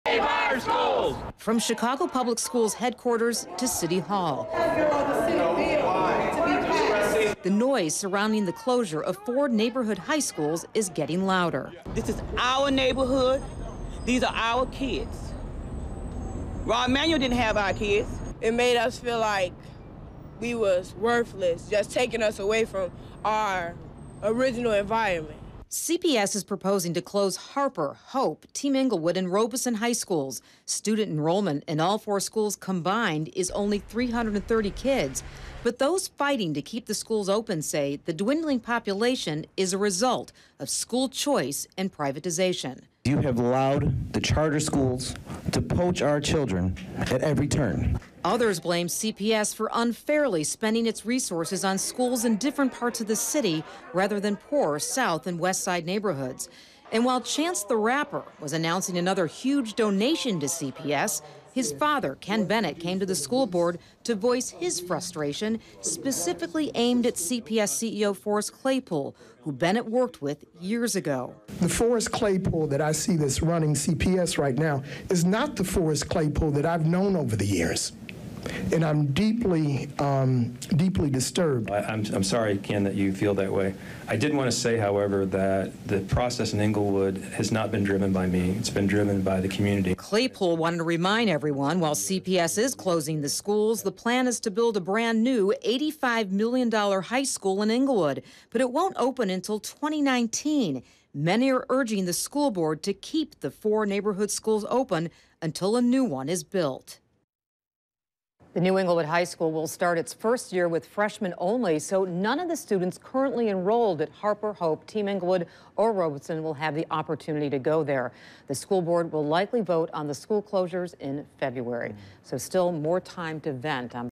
CPS proposal would close 4 schools in Englewood
A Chicago Public Schools proposal would shutter four high schools in the city's Englewood neighborhood.